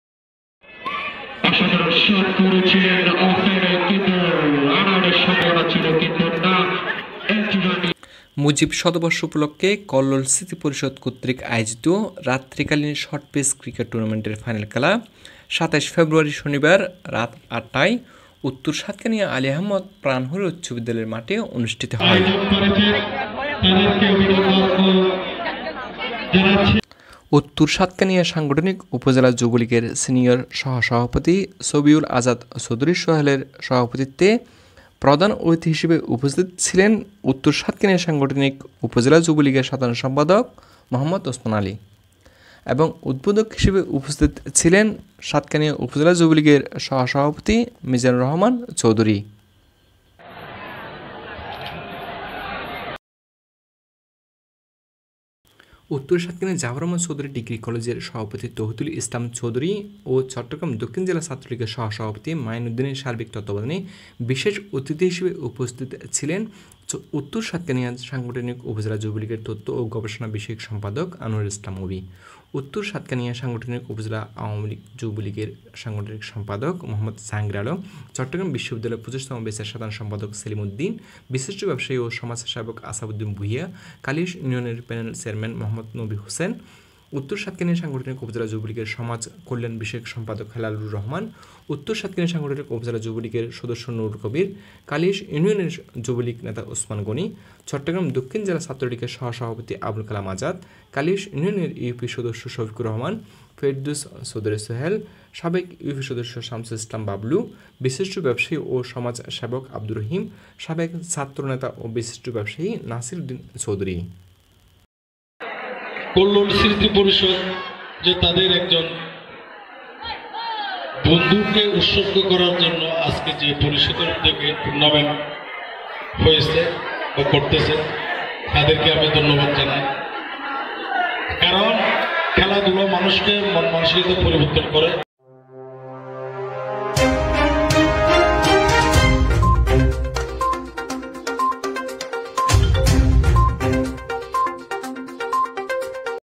''ক্রীড়াই শক্তি ক্রীড়াই বল, মাদক ছেড়ে মাঠে চল।" সাতকানিয়া কল্লোল স্মৃতি পরিষদের ক্রিকেট টুর্ণামেন্ট।
সাতকানিয়ায় মুজিব শতবর্ষ উপলক্ষে কল্লোল স্মৃতি পরিষদের ক্রিকেট টুর্ণামেন্টের ফাইনাল খেলা অনুষ্ঠিত

“ক্রীড়াই শক্তি ক্রীড়াই বল, মাদক ছেড়ে মাঠে চল।” এই প্রতিপাদ্য নিয়ে কল্লোল স্মৃতি পরিষদ কর্তৃক আয়োজিত মুজিব শতবর্ষ উপলক্ষে ১ম বারের মত আয়োজিত রাত্রিকালীন শর্টপিচ ক্রিকেট টুর্ণামেন্টের ফাইনাল খেলা ২৭ ফেব্রুয়ারি (শনিবার) রাত ৮ টায় উত্তর সাতকানিয়া আলী আহমদ প্রাণ হরি উচ্চ বিদ্যালয়ের মাঠে অনুষ্ঠিত হয়।
উত্তর সাতকানিয়া সাংগঠনিক উপজেলা যুবলীগের সিনিয়র সহ সভাপতি সবিউল আজাদ চৌধুরী সোহেলের সভাপতিত্বে প্রধান অতিথি হিসেবে উপস্থিত ছিলেন উত্তর সাতকানিয়া সাংগঠনিক উপজেলা যুবলীগের সাধারণ সম্পাদক মোহাম্মদ ওচমান আলী এবং উদ্বোধক হিসেবে উপস্থিত ছিলেন সাতকানিয়া উপজেলা কৃষকলীগের সহ সভাপতি মিজানুর রহমান চৌধুরী।
বিশেষ অতিথি হিসেবে উপস্থিত ছিলেন, উত্তর সাতকানিয়া সাংগঠনিক উপজেলা যুবলীগের তথ্য ও গবেষণা বিষয়ক সম্পাদক আনোয়ারুল ইসলাম অভি, উত্তর সাতকানিয়া সাংগঠনিক উপজেলা আওয়ামী যুবলীগের সাংগঠনিক সম্পাদক মোঃ জাহাঙ্গীর আলম, চট্টগ্রাম বিশ্ববিদ্যালয় ২৫ তম ব্যাচের সাধারণ সম্পাদক সেলিম উদ্দীন, বিশিষ্ট ব্যবসায়ী ও সমাজ সেবক আসহাব উদ্দীন ভুইঁয়া, কালিয়াইশ ইউনিয়নের প্যানেল চেয়ারম্যান মোঃ নবী হোসেন, উত্তর সাতকানিয়া সাংগঠনিক উপজেলা যুবলীগের সমাজ কল্যাণ বিষয়ক সম্পাদক হেলালুর রহমান, উত্তর সাতকানিয়া সাংগঠনিক উপজেলা যুবলীগের সদস্য নুরুল কবির, কালিয়াইশ ইউনিয়নের যুবলীগ নেতা ওসমান গণি, চট্টগ্রাম দক্ষিণ জেলা ছাত্রলীগের সহ সভাপতি আবুল কালাম আজাদ, কালিয়াইশ ইউনিয়নের ইউপি সদস্য শফিকুর রহমান, ফেরদৌস চৌধুরী সোহেল, সাবেক ইউপি সদস্য সামশুল ইসলাম বাবলু, বিশিষ্ট ব্যবসায়ী ও সমাজ সেবক আবদুর রহিম, সাবেক ছাত্র নেতা ও বিশিষ্ট ব্যবসায়ী নাছির উদ্দীন চৌধুরী।
এছাড়াও সার্বিক তত্তাবধানে ছিলেন উত্তর সাতকানিয়া জাফর আহমদ চৌধুরী ডিগ্রি কলেজের সভাপতি তৌহিদুল ইসলাম চৌধুরী, চট্টগ্রাম দক্ষিণ জেলা ছাত্রলীগের সহ সভপতি মাঈন উদ্দীন, ছাত্রলীগ নেতা আরফাত এবং ধারাভাষ্যে ছিলেন জহির।
উক্ত খেলায় চ্যাম্পিয়ন হয় কালিয়াইশ মাস্টার হাট একতা সংঘ ও রার্নাস আপ মরহুম জাফর আহমদ চৌধুরী একতা সংঘ।